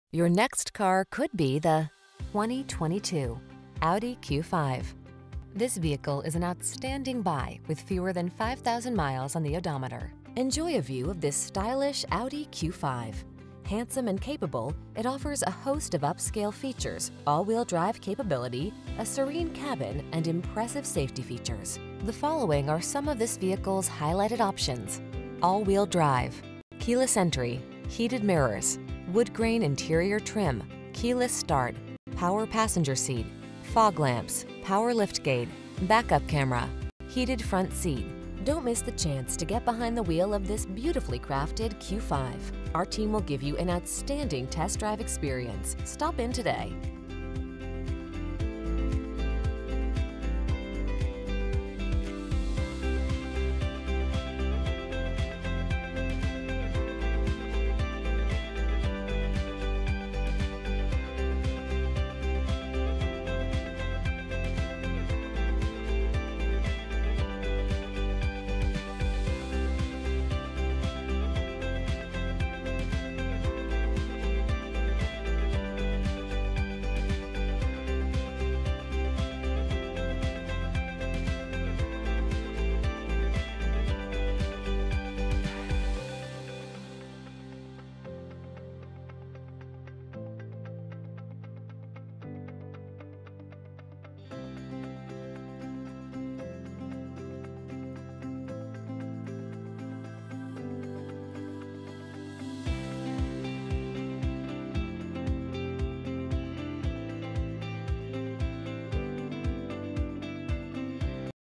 2022 Audi Q5 Lakeland, Plant City, Winter Haven, FL N9N15125B
2022 Audi Q5 S line Premium - Stock#: N9N15125B - VIN#: WA1GAAFY6N2087410

ONLY 3,690 Miles! FUEL EFFICIENT 28 MPG Hwy/23 MPG City!